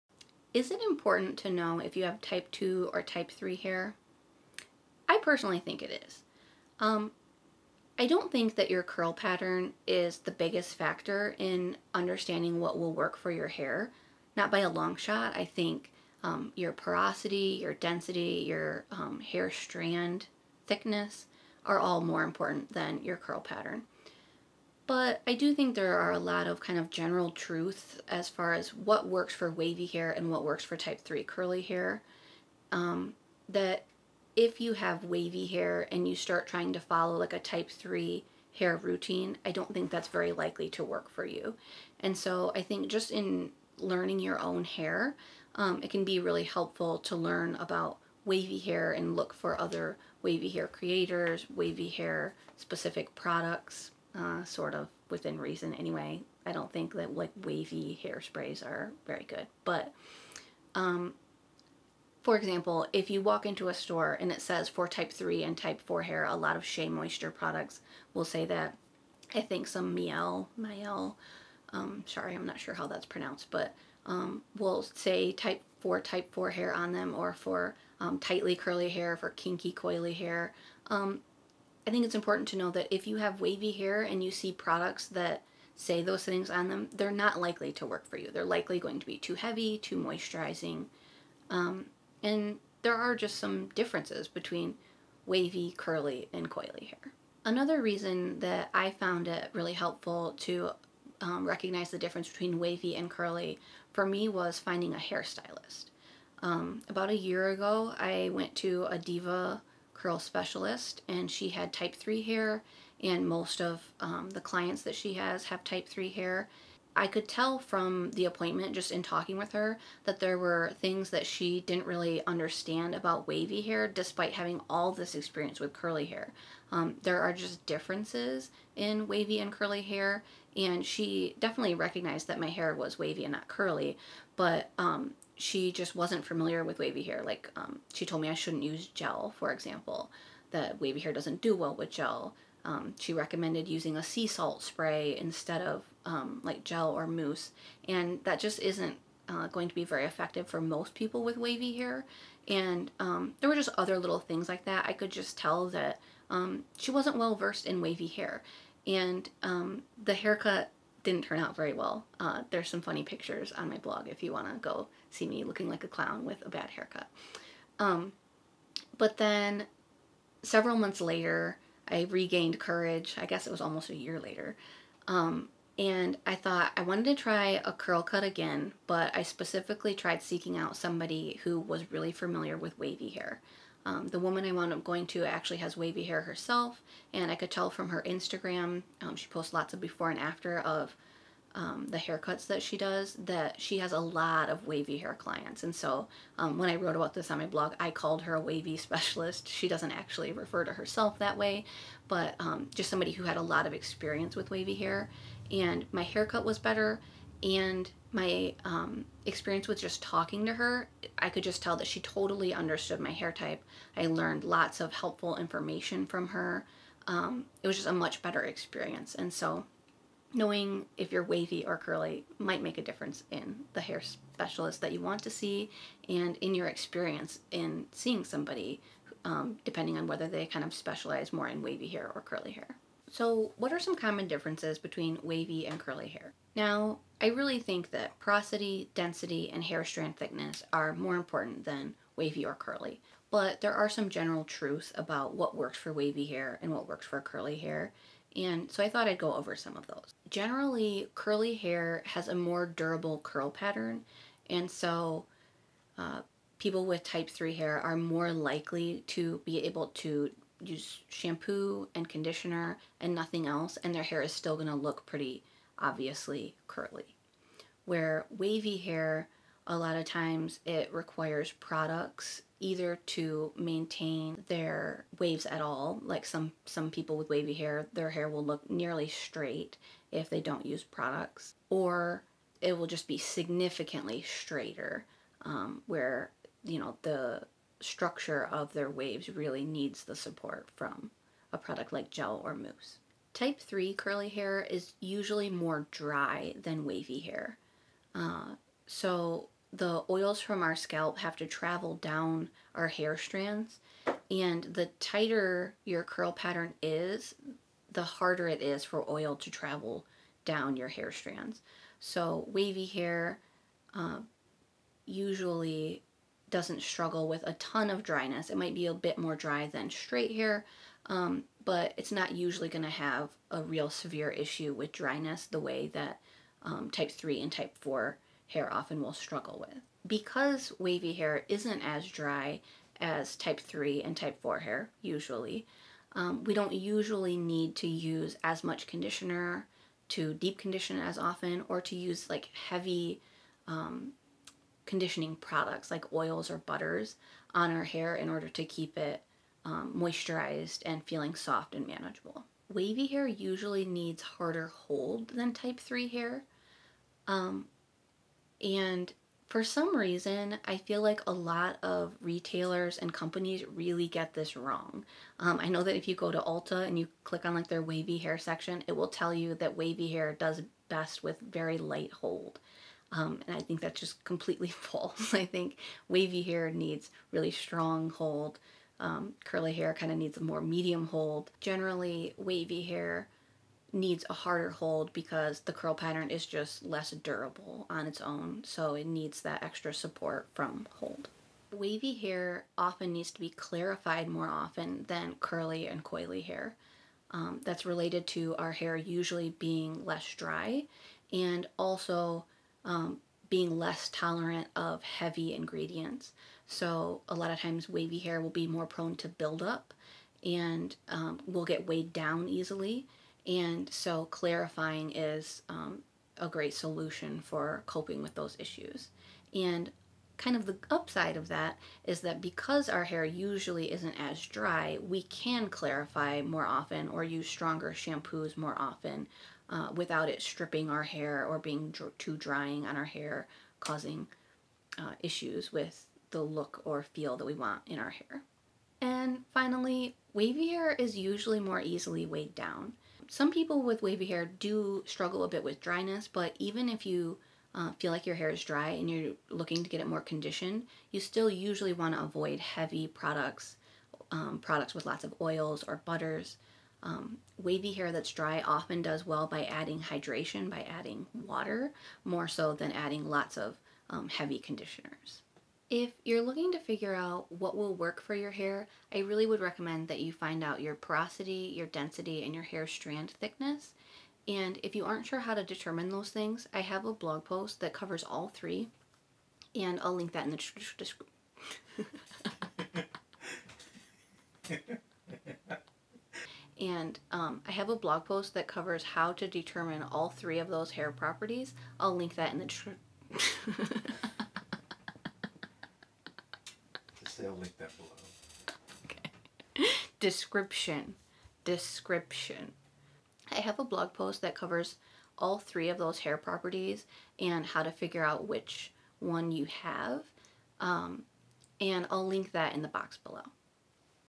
Common Differences Between Wavy And Curly Hair Types
Does it matter if your hair is wavy or curly? I think it does. While other hair properties may matter more, here are common differences between wavy and curly hair. In this video I cover what some of those common differences are.

My blog post sharing my type 3 curly devacut experience (clown photo included 🤡)

My blog post sharing my experience seeing a wavy specialist for a devacut.